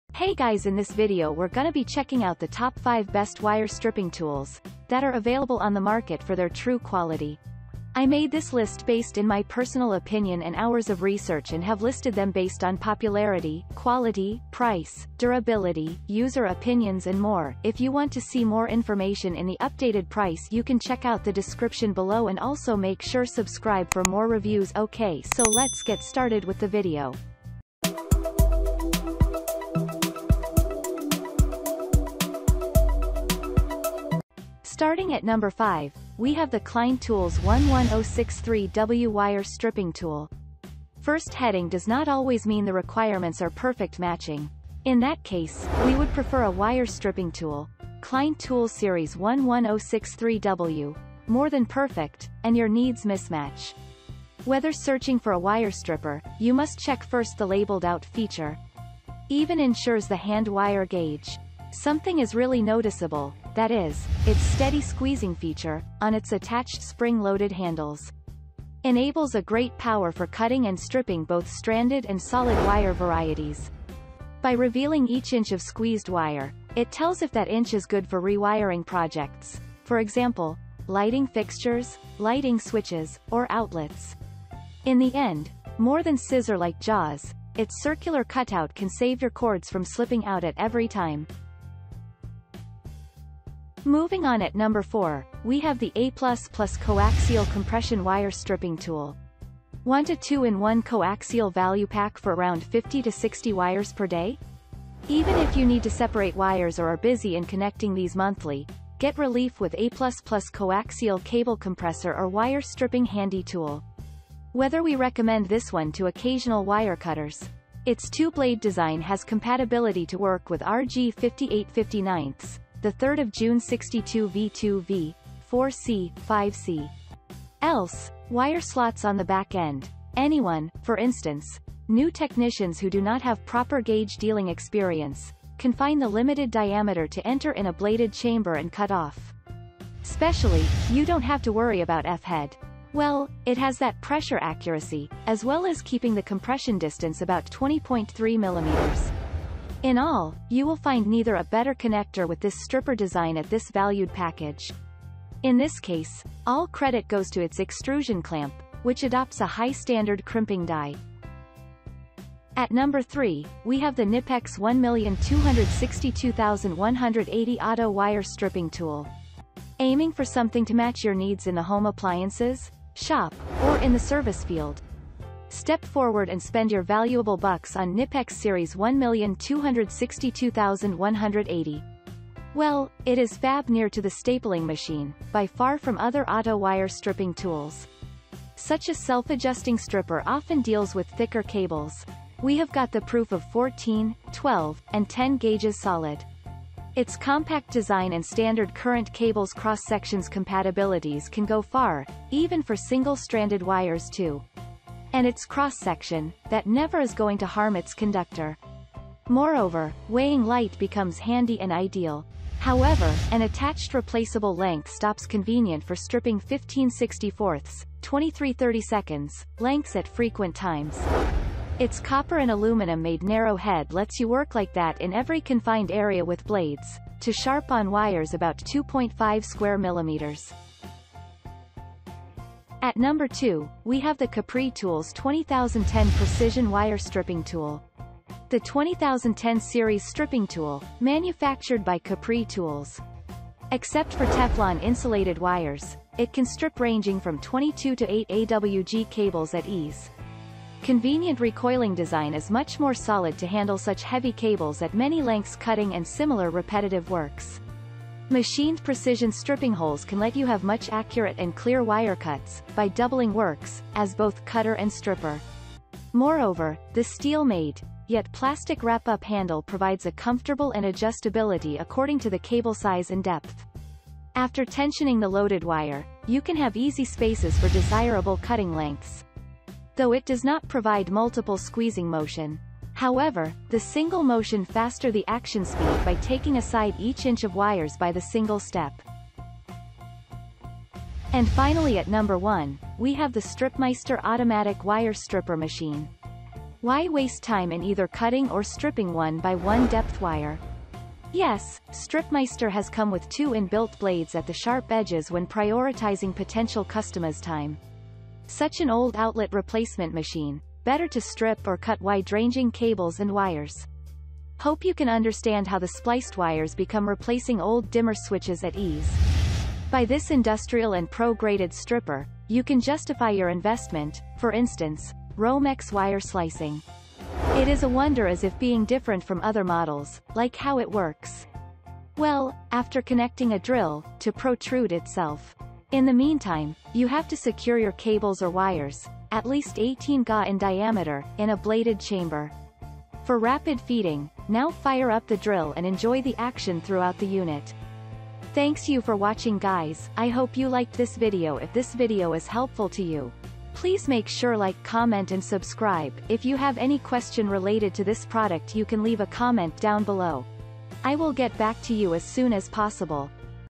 Top 5 Best Wire Stripping Tools for 2021
► Links to the Best Wire Stripping Tools we listed in this video:

🔹5.  Klein Tools 11063W Wire Stripping Tool

🔹4.  APlus+ Coaxial Compression Wire Stripping Tool

🔹3.   Knipex 1262180 Auto Wire Stripping Tool

🔹2.  Capri Tools 20010 Precision Wire Stripping Tool

🔹1.  StripMeister Automatic Wire Stripper Machine